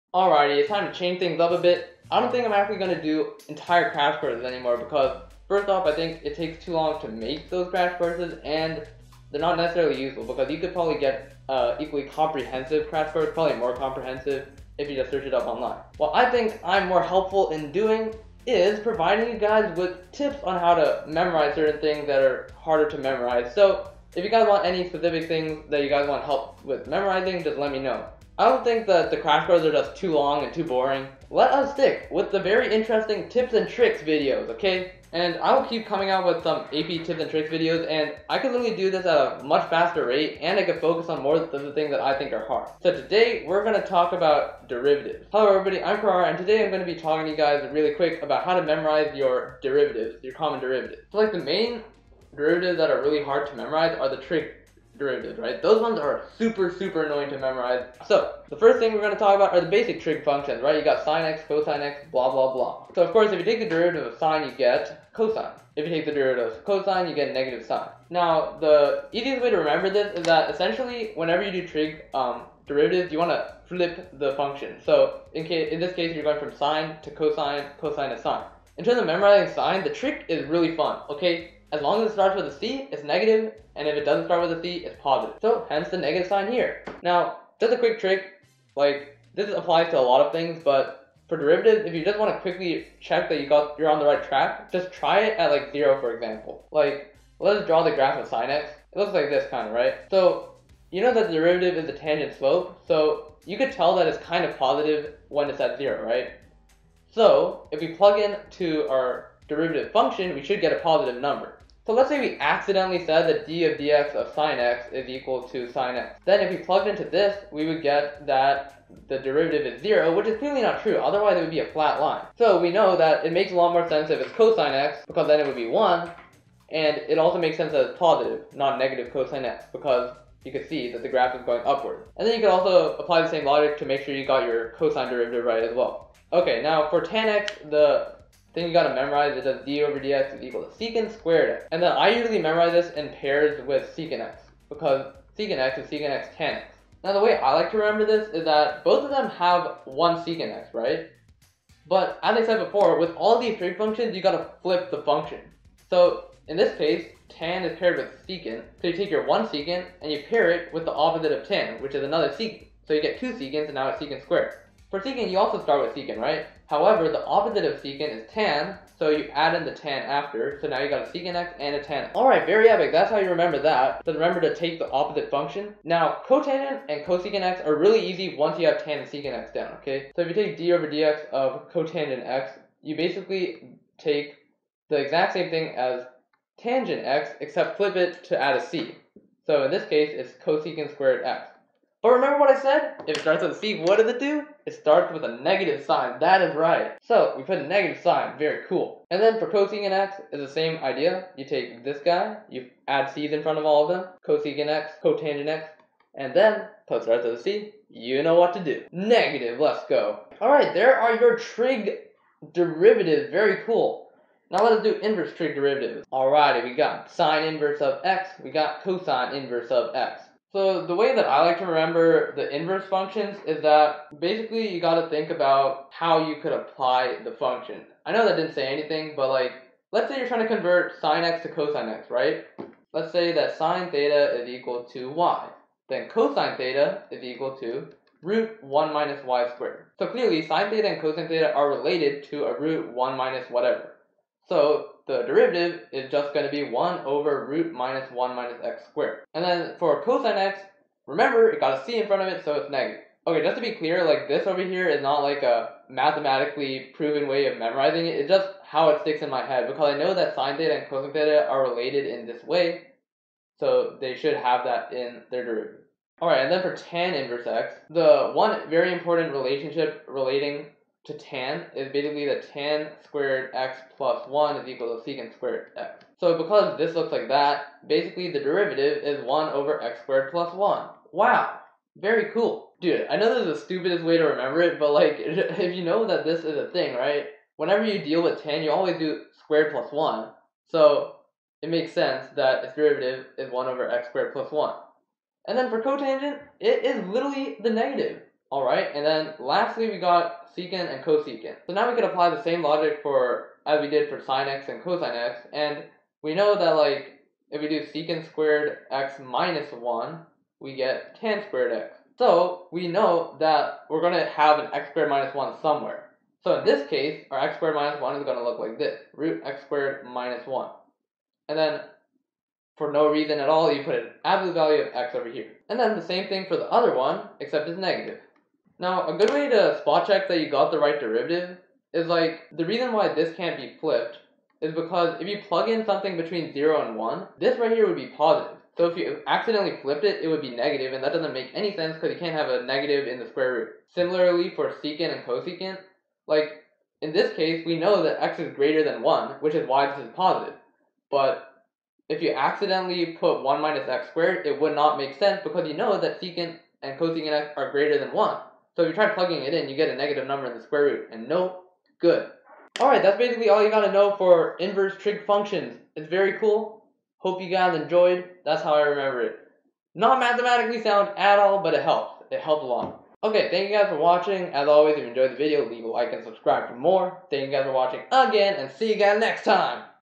AP Calculus BC Tips: Memorizing Trig and Inverse Trig Derivatives!!!
Here's the way I remember trig and inverse trig functions :D. I literally cannot forget them anymore because of the really dumb associations I made in my head, but hey, it works!